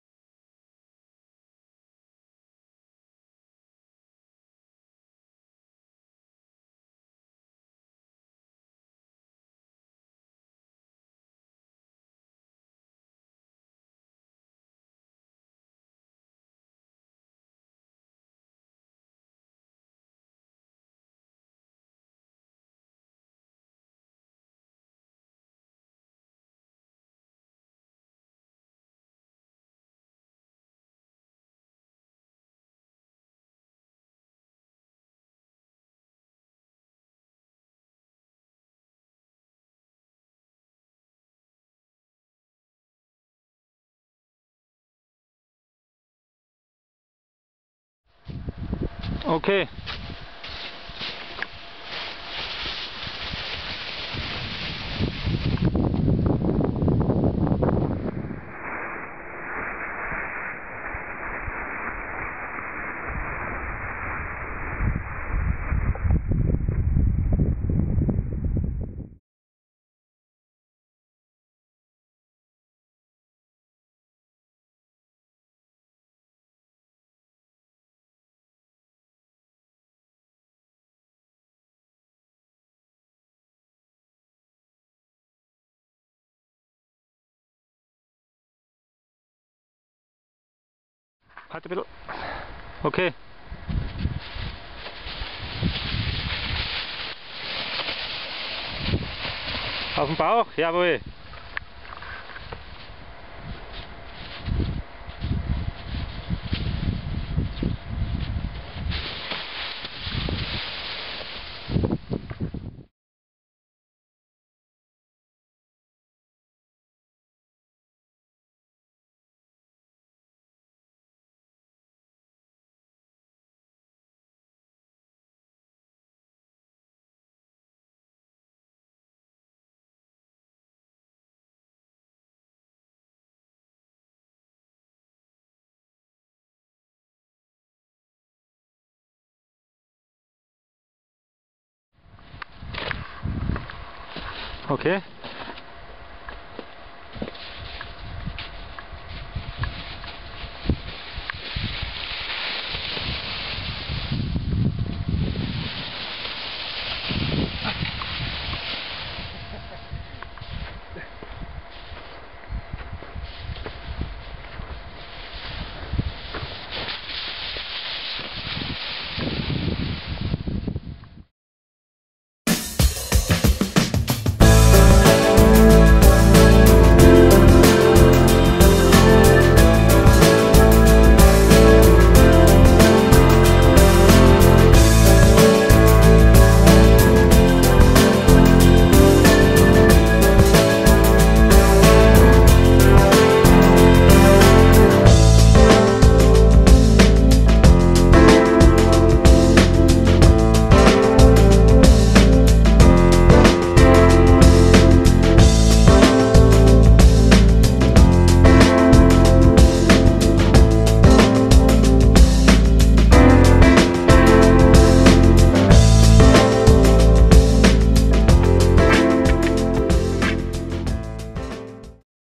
Sicherheit am Berg "Bremsen von Rutschen" - outdoor-life -
Möglichkeiten Abstürze im steilen Firn zu verhindern. Diese Aufnahmen sind im Jahr 2010 im Rahmen einer Ausbildung beim DAV Summit Club entstanden und wurden von einem Bergführer angeleitet.